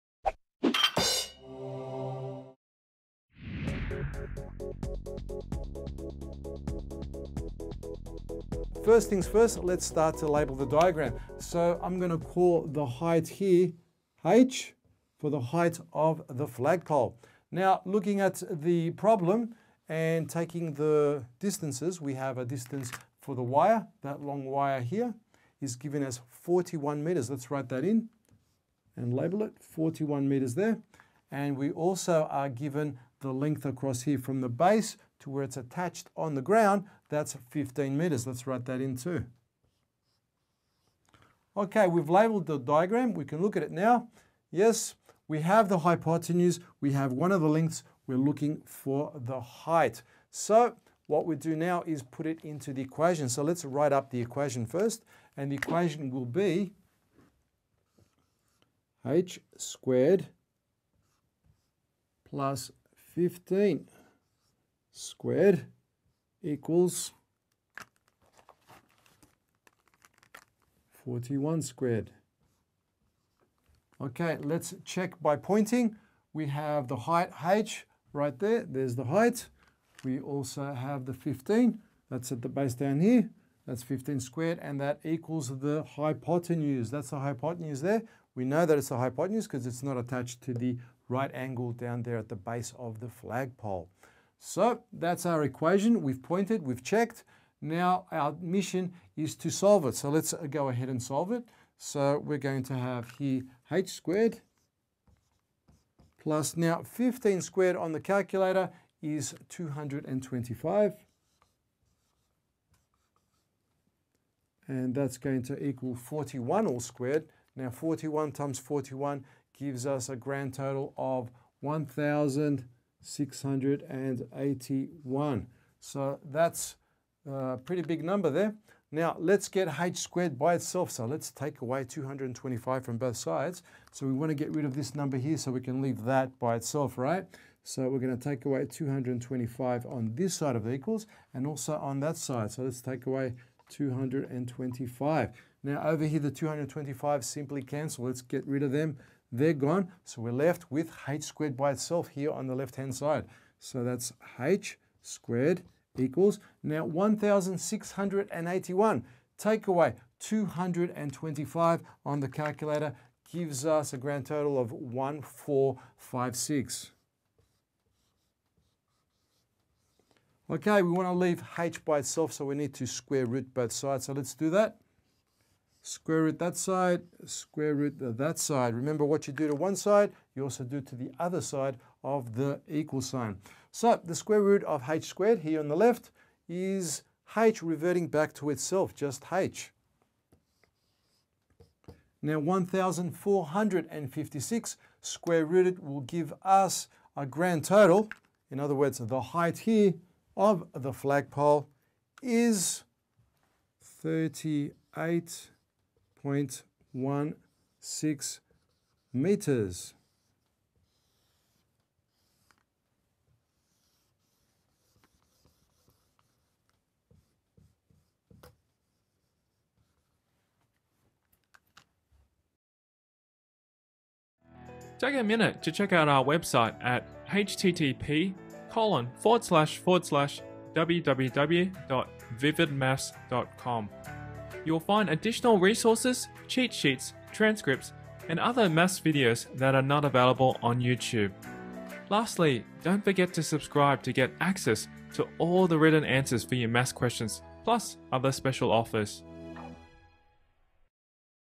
Pythagoras Theorem: Find the Shorter Side: Word Problem - VividMath.com
Use Pythagoras' Theorem to find the Shorter Side of a Right Angled Triangle (Word Problem).